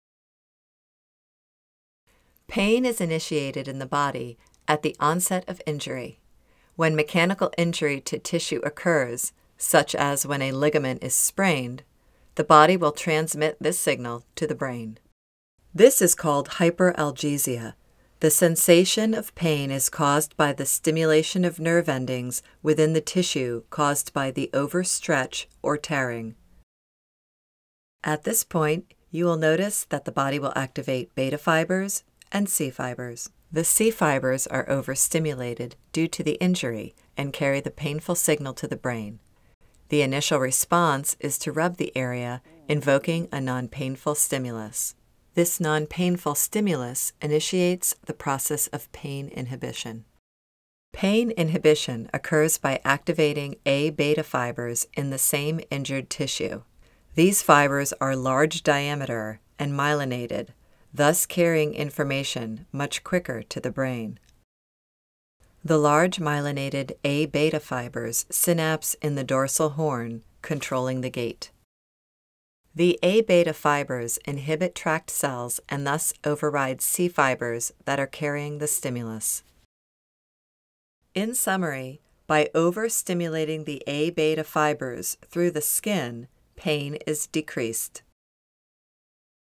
Pain Modulation
Animations for the online resources accompanying the 2012 Starkey,Therapeutic Modalities book